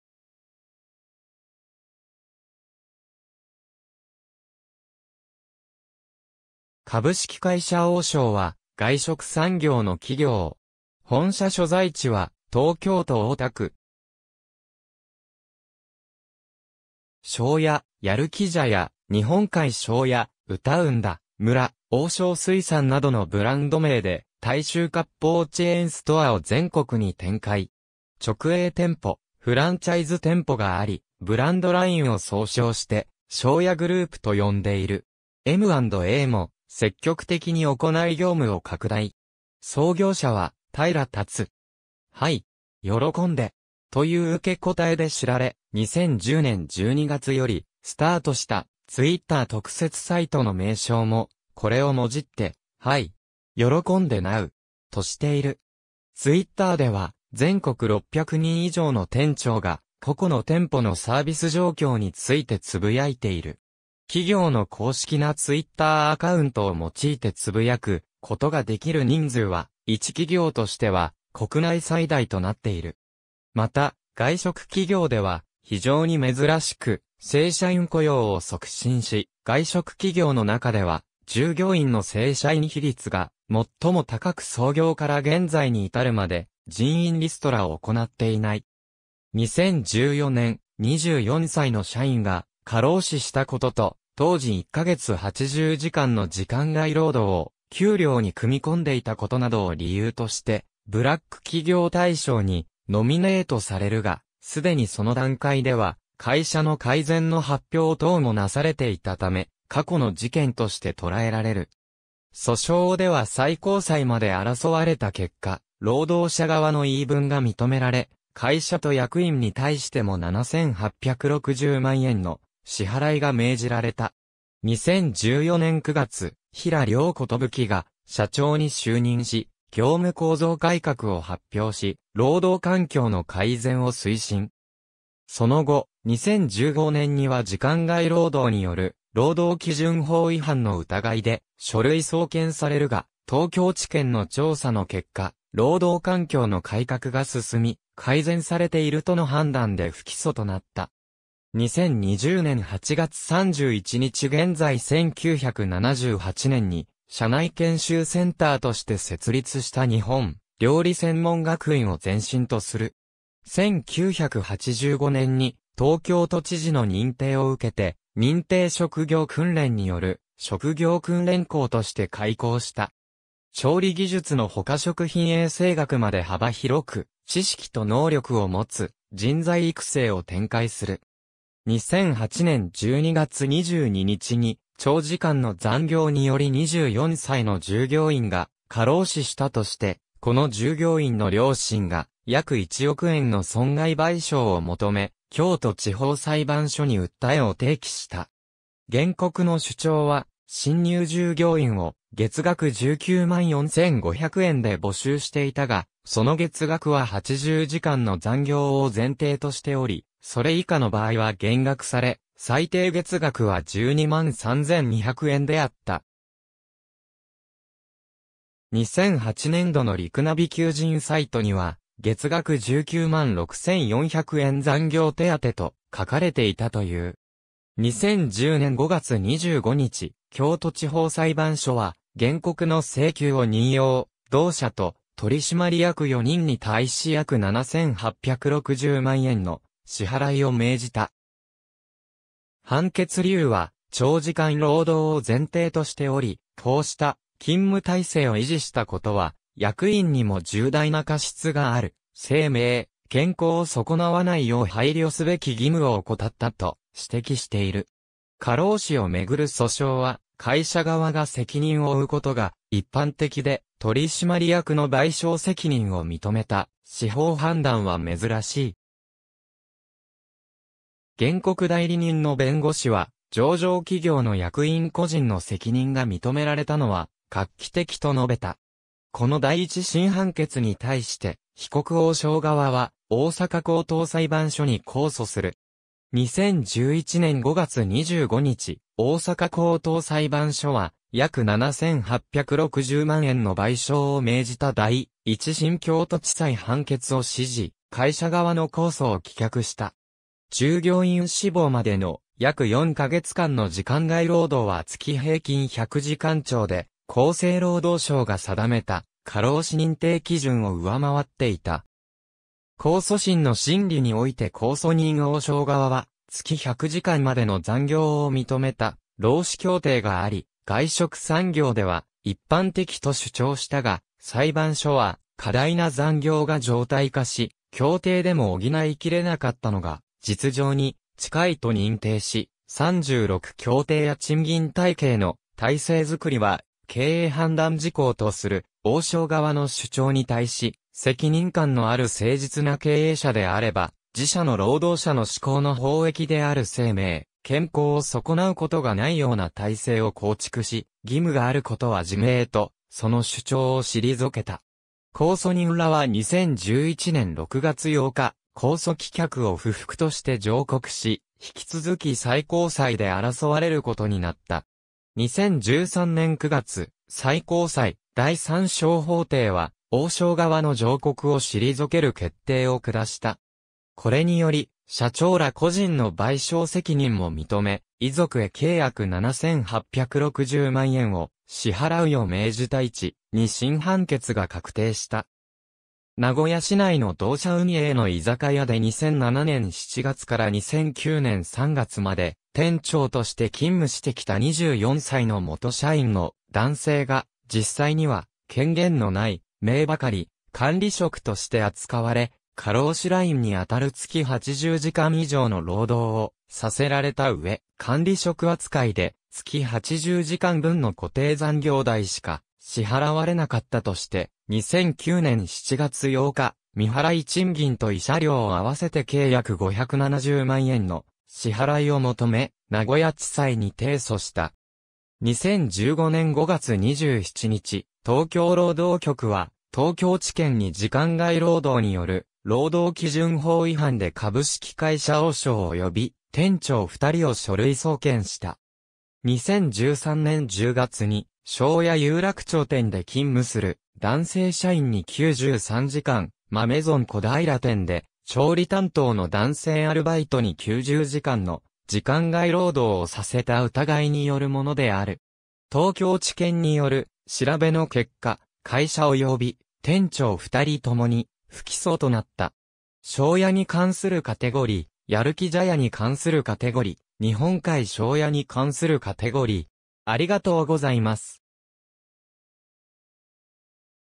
大庄
株式会社大庄（だいしょう、英: DAISYO CORPORATION）は、外食産業の企業。
本社所在地は東京都大田区。
『庄や』、『やるき茶屋』、『日本海庄や』、『うたうんだ村』、『大庄水産』などのブランド名で大衆割烹チェーンストアを全国に展開。
直営店舗、フランチャイズ店舗があり、ブランドラインを総称して「庄やグループ」と呼んでいる。
M&Aも積極的に行い業務を拡大。
創業者は平辰。
「はい!よろこんで！」という受け答えで知られ、2010年12月よりスタートしたTwitter特設サイトの名称も、これをもじって「ハイ！ よろこんでナウ！」としている。
Twitterでは全国600人以上の店長が個々の店舗のサービス状況についてつぶやいている。
企業の公式なTwitterアカウントを用いてつぶやくことができる人数は、一企業としては国内最大となっている。
また、外食企業では非常に珍しく、正社員雇用を促進し外食企業の中では従業員の正社員比率が最も高く創業から現在に至るまで人員リストラを行っていない。
2014年、24歳の社員が過労死したことと当時1ヶ月80時間の時間外労働を給料に組み込んでいたこと（80時間の時間外労働を行わないと満額の給料が支払われない）などを理由としてブラック企業大賞にノミネートされるが、既にその段階では会社の改善の発表等もなされていたため、過去の事件として捉えられる。
訴訟では最高裁まで争われた結果、労働者側の言い分が認められ、会社と役員に対しても7860万円の支払いが命じられた。
2014年9月平了寿が社長に就任し、業務構造改革を発表し労働環境の改善を推進。
その後、2015年には時間外労働による労働基準法違反の疑いで書類送検されるが、東京地検の調査の結果、労働環境の改革が進み「改善されている」との判断で不起訴となった。
2020年8月31日現在 1978年に社内研修センターとして設立した日本料理専門学院を前身とする。
1985年に東京都知事の認定を受けて認定職業訓練による職業訓練校（現・職業能力開発校）として開校した。
調理技術のほか食品衛生学まで幅広く知識と能力をもつ人材育成を展開する。
2008年12月22日に、長時間の残業により24歳の従業員が過労死したとして、この従業員の両親が、約1億円の損害賠償を求め、京都地方裁判所に訴えを提起した。
原告の主張は、新入従業員を月額19万4,500円で募集していたが、その月額は80時間の残業を前提としており、それ以下の場合は減額され、最低月額は12万3,200円であった。
2008年度のリクナビ求人サイトには、月額19万6,400円＋残業手当と書かれていたという。
2010年5月25日、京都地方裁判所は原告の請求を認容、同社と取締役4人に対し約7,860万円の支払いを命じた。
判決理由は「長時間労働を前提としており、こうした勤務体制を維持したことは、役員にも重大な過失がある」、「生命、健康を損なわないよう配慮すべき義務を怠った」と指摘している。
過労死を巡る訴訟は、会社側が責任を負うことが一般的で、取締役の賠償責任を認めた司法判断は珍しい。
原告代理人の弁護士は、「上場企業の役員個人の責任が認められたのは画期的」と述べた。
この第一審判決に対して、被告大庄側は大阪高等裁判所に控訴する。
2011年5月25日、大阪高等裁判所は約7,860万円の賠償を命じた第一審京都地裁判決を支持、会社側の控訴を棄却した。
従業員死亡までの約4カ月間の時間外労働は月平均100時間超で、厚生労働省が定めた過労死認定基準（月80時間超）を上回っていた。
控訴審の審理において控訴人大庄側は、月100時間までの残業を認めた労使協定があり「外食産業では一般的」と主張したが、裁判所は「過大な残業が常態化し、協定でも補いきれなかったのが実情に近い」と認定し、三六協定や賃金体系の体制作りは「経営判断事項」とする大庄側の主張に対し、「責任感のある誠実な経営者であれば、自社の労働者の至高の法益である生命・健康を損なうことがないような体制を構築し、（中略）義務があることは自明」とその主張を退けた。
控訴人（会社側）らは2011年6月8日、控訴棄却を不服として上告し、引き続き最高裁で争われることになった。
2013年9月、最高裁第3小法廷は大庄側の上告を退ける決定を下した。
これにより社長ら個人の賠償責任も認め、遺族へ計約7860万円を...